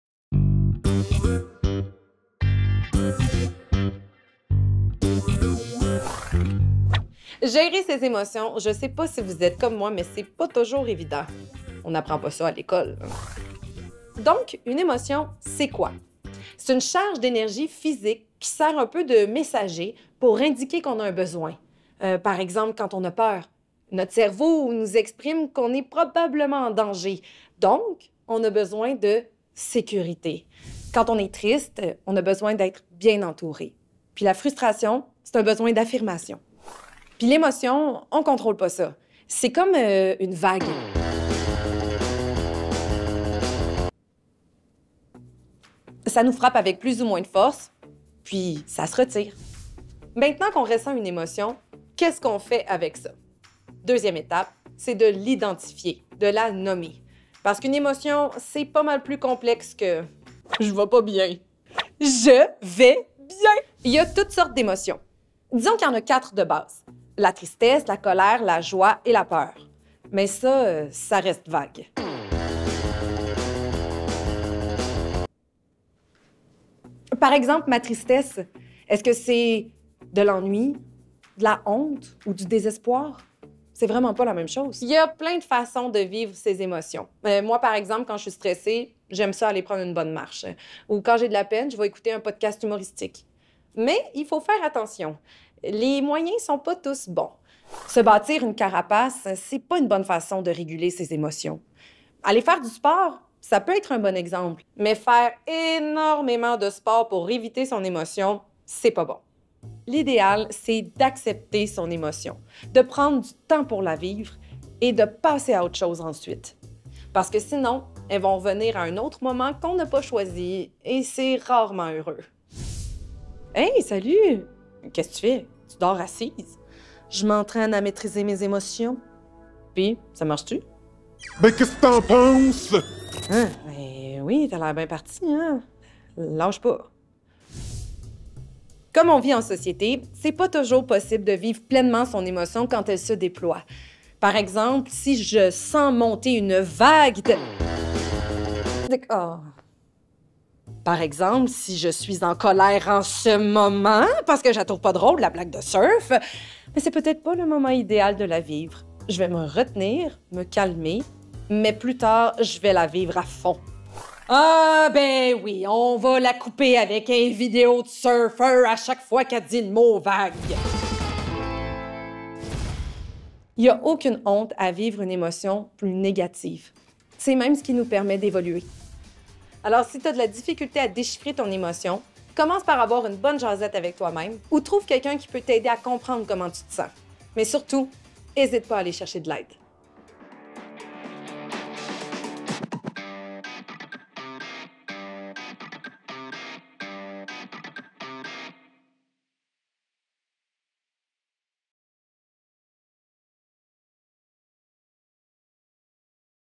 "Gérer les vagues d'émotion" avec Pascale Marineau | Station SMÉ (5/7)
Santé mentale, santé mentale, santé mentale... deux mots sur toutes les bouches mais sait-on vraiment comme gérer *ça* ?

À l'ÉNH, on essaie de paver le chemin vers plus d'empathie en termes d'enjeux de santé mentale.

Avec ISMÉES (Initiative sur la Santé Mentale En Enseignement Supérieur), nous avons réalisé 7 capsules vidéo humoristiques pour défaire mythes et préconceptions liés à la santé mentale avec une touche humoristique.

Le contenu a été savamment élaboré en partenariat avec des professionnels de la santé mentale afin de développer la littératie sur la santé mentale auprès de la population aux études postsecondaires et amener les étudiant.es à se questionner sur leur comportement et à mieux comprendre ce qui est sain pour leur santé mentale afin de se mettre en action (autogestion) pour prévenir l’apparition de difficultés.

L’utilisation de l’humour, le format dynamique des capsules et les exemples liés à leur réalité garantissent un impact plus grand auprès de la clientèle étudiante.

Cette série de vidéos a été réalisée par notre diplômé Patrick Guérard.